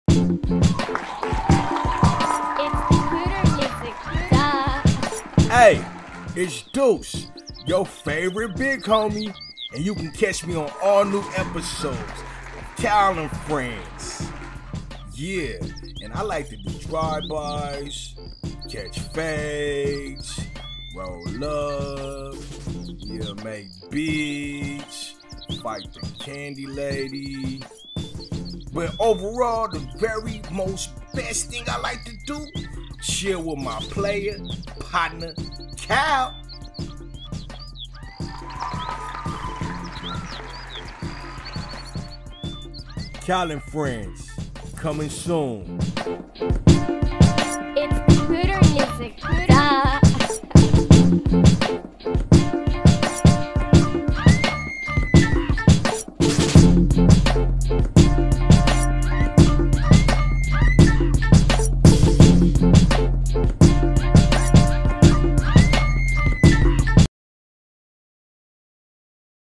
😎😤 CAL & FRIENDS™ INTRODUCING: DUCE
GET READY for one of the most entertaining cartoons - that could EVER go viral! 😲

But 1st - we want to introduce "DUCE" - a cool dude from the world of CAL & FRIENDS!